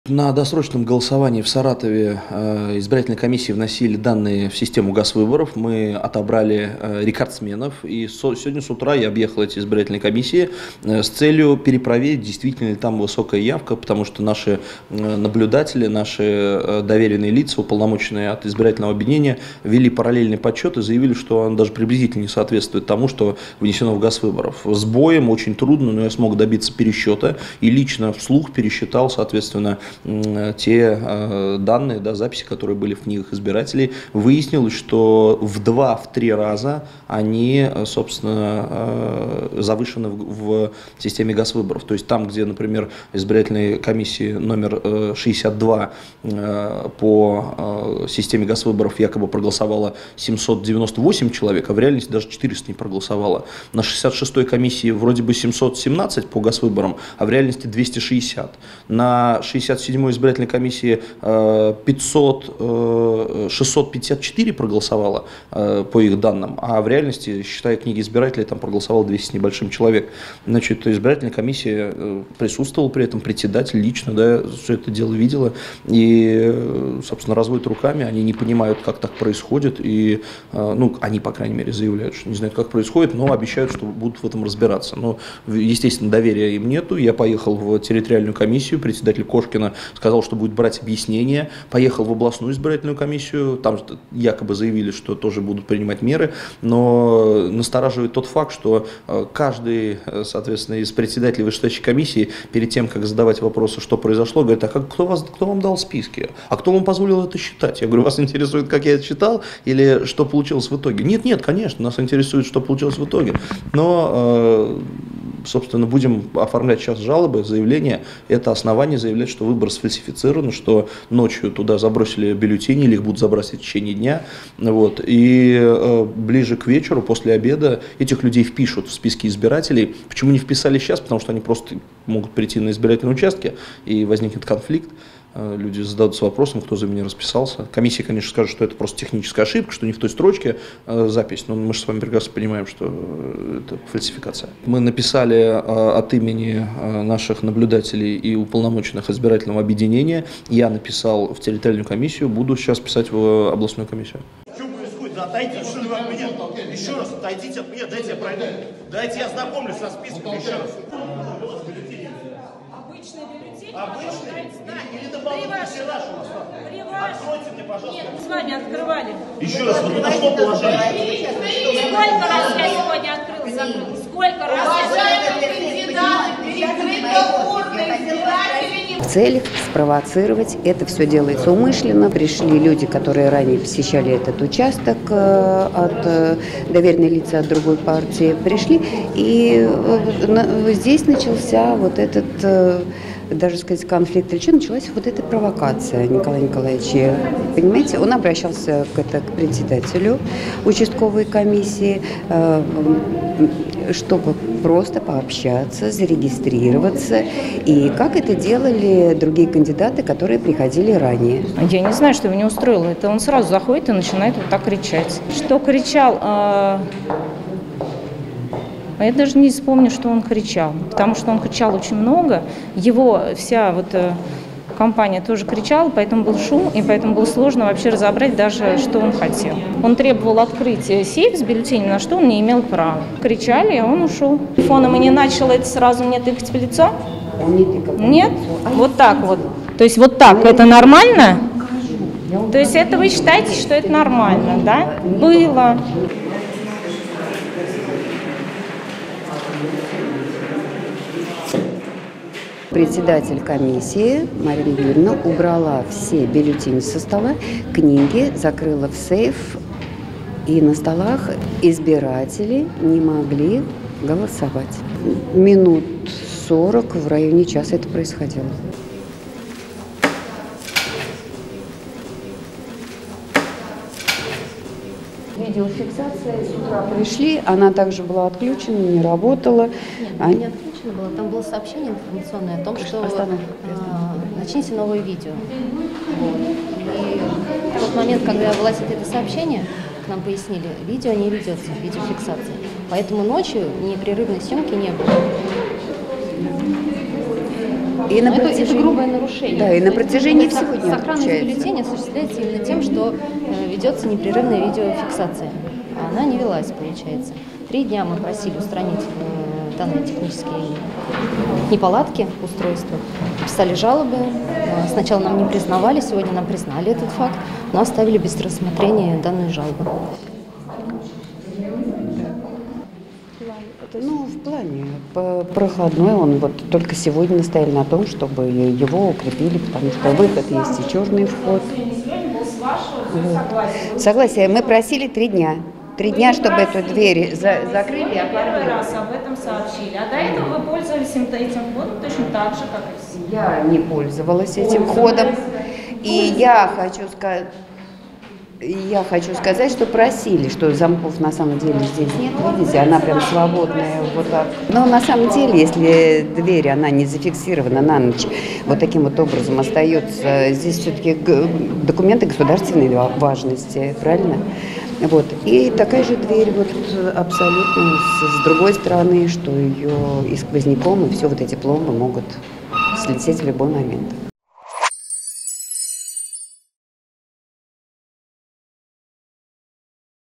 Николай Бондаренко добился пересчёта явки
Кандидат в депутаты Саратовской областной думы добился пересчета количества избирателей на участках №№62, 66, 67.
Накануне его соратники изучили данные ГАС «Выборы» и отобрали участки с наибольшей явкой. Работавшие на этих УИК наблюдатели вели параллельный подсчет, и, по их данным, на голосование пришло в 2,5-3 раза меньше человек, чем заявлено в системе.
Днем ранее, 10 сентября, на УИК №47 доверенные лица со стороны кандидата в депутаты Саратовской областной думы от «Единой России» Михаила Исаева, среди которых был силач Вячеслав Максюта, с шумом не позволили Бондаренко проверить список избирателей и исключить факт возможных вбросов. Из-за этого инцидента был приостановлен процесс голосования.
Кроме того, наблюдатели УИК №47 обратили внимание на то, что на участке отключалось видеонаблюдение, а двери в помещение для голосования были не закрыты. В том числе и ночью.